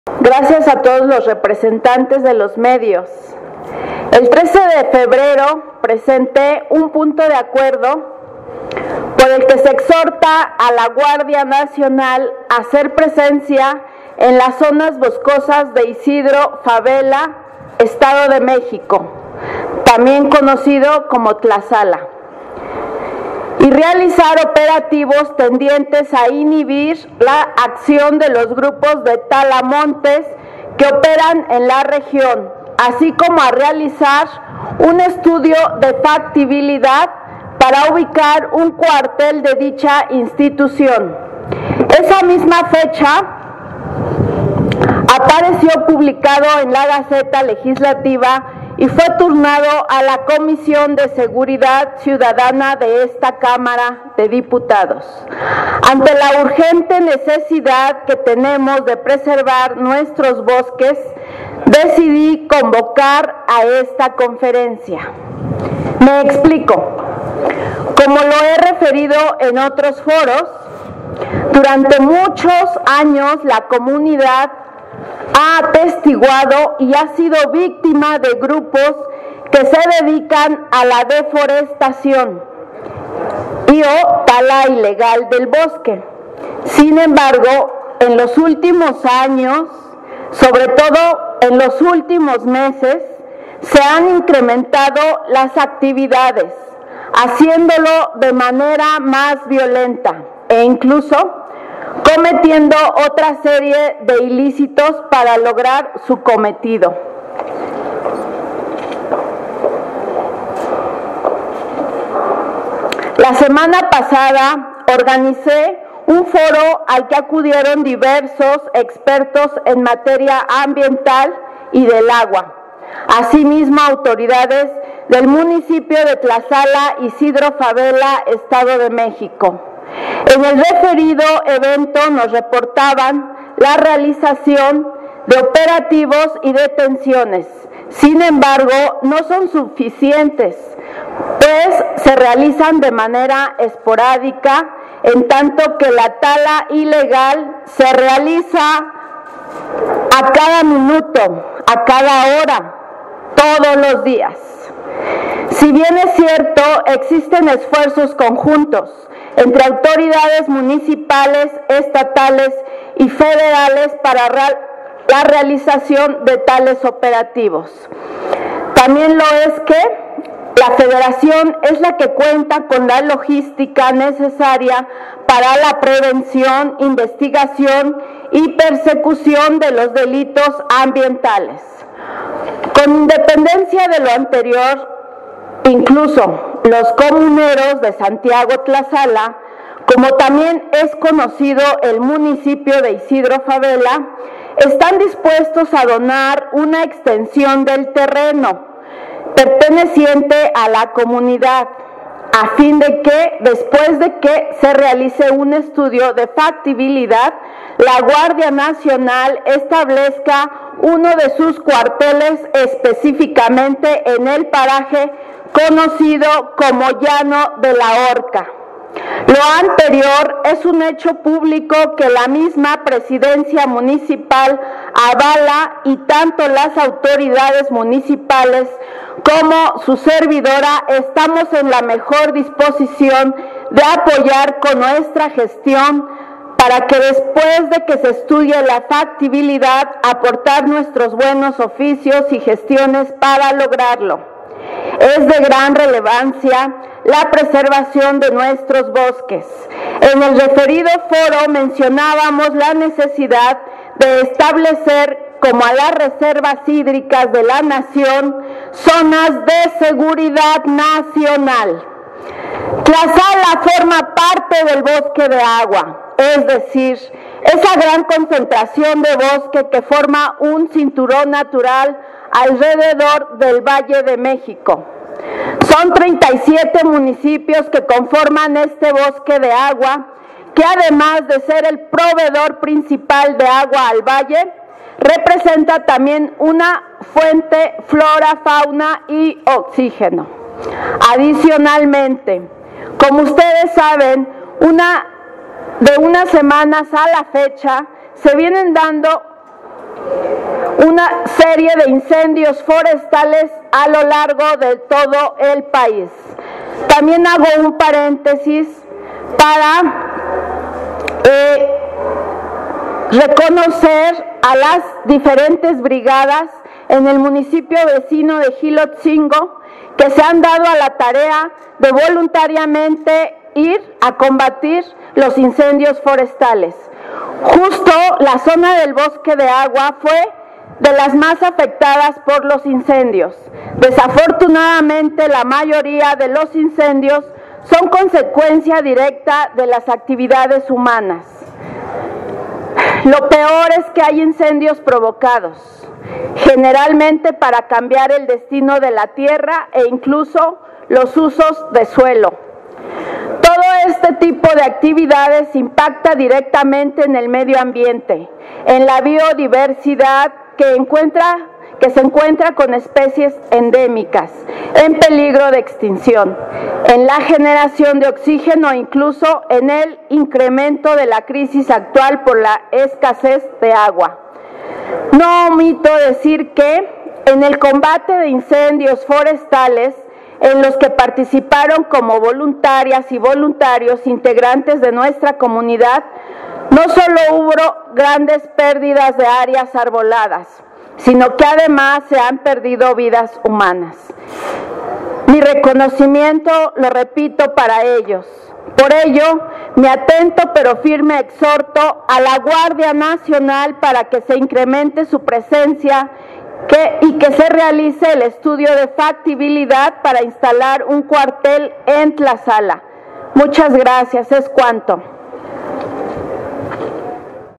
Conferencia de prensa | Dip. Ma. Cristina Vargas Osnaya | Morena | 17/04/2024
Impulsa María Cristina Vargas exhorto a la GN para realizar operativos que inhiban a grupos de talamontes en Isidro Fabela, Estado de México

⚫La diputada de Morena pide realizar un estudio de factibilidad para ubicar un cuartel de dicha institución en esa zona

Palacio Legislativo, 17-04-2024 (Notilegis).- La diputada María Cristina Vargas Osnaya (Morena) informó que impulsa un punto de acuerdo para exhortar a la Guardia Nacional (GN) a hacer presencia en las zonas boscosas de Isidro Fabela, Estado de México, y realizar operativos para inhibir la acción de los grupos de talamontes que operan en la región.

Asimismo, pidió realizar un estudio de factibilidad para ubicar un cuartel de dicha institución en esa zona.

En conferencia de prensa, indicó que el documento lo presentó el pasado 13 de febrero, el cual fue turnado a la Comisión de Seguridad Ciudadana.

Vargas Osnaya recordó que durante muchos años la comunidad ha atestiguado y ha sido víctima de grupos que se dedican a la deforestación y a la tala ilegal del bosque; sin embargo, abundó que “en los últimos años, sobre todo en los últimos meses, se han incrementado las actividades, haciéndolo de manera más violenta e incluso cometiendo otra serie de ilícitos para lograr su cometido”.

Expuso que la semana pasada realizó un foro al que acudieron diversos expertos en materia ambiental y del agua, así como autoridades del municipio de Tlazala, Isidro Fabela, Estado de México, donde reportaron la realización de operativos y detenciones, sin embargo, dijo “no son suficientes, pues se realizan de manera esporádica en tanto que la tala ilegal se realiza todos los días”.

Destacó que existen esfuerzos conjuntos entre autoridades municipales, estatales y federales para la realización de operativos, particularmente la Federación es la que cuenta con la logística necesaria para la prevención, investigación y persecución de los delitos ambientales.

Agregó que en el foro se habló de la necesidad de establecer a las reservas hídricas de la nación zonas de seguridad nacional, ya que Tlazala, forma parte de los 37 municipios que conforman el bosque de agua que además de ser el proveedor principal de agua al valle, representa también una fuente, flora, fauna y oxígeno.

La diputada aseguró que los comuneros de Santiago Tlazala están dispuestos a donar una extensión del terreno perteneciente a la comunidad, a fin de que se establezca uno de los cuarteles de la GN en el paraje conocido como “Llano de la Orca”, después de que se realice un estudio de factibilidad.

“Lo anterior es un hecho público que la misma presidencia municipal avala y tanto las autoridades municipales como su servidora estamos en la mejor disposición de apoyar con nuestra gestión, aportar nuestros buenos oficios y gestiones para lograrlo, es de gran relevancia la preservación de nuestros bosques”, puntualizó.

En otro tema, Vargas Osnaya comentó que de unas semanas a la fecha se vienen dando una serie de incendios forestales a lo largo de todo el país, ante ello reconoció a las diferentes brigadas en el municipio de Jilotzingo que se han dado a la tarea de voluntariamente ir a combatir los incendios forestales, justo en la zona del bosque de agua que fue de las más afectadas por los incendios.

Desafortunadamente, explicó, la mayoría de los incendios son consecuencia directa de las actividades humanas, “lo peor es que hay incendios provocados, generalmente para cambiar el destino de la tierra e incluso los usos de suelo.

“Todo este tipo de actividades impacta directamente en el medio ambiente, en la biodiversidad que se encuentra con especies endémicas, en peligro de extinción, en la generación de oxígeno, incluso en el incremento de la crisis actual por la escasez de agua”, señaló.

EGV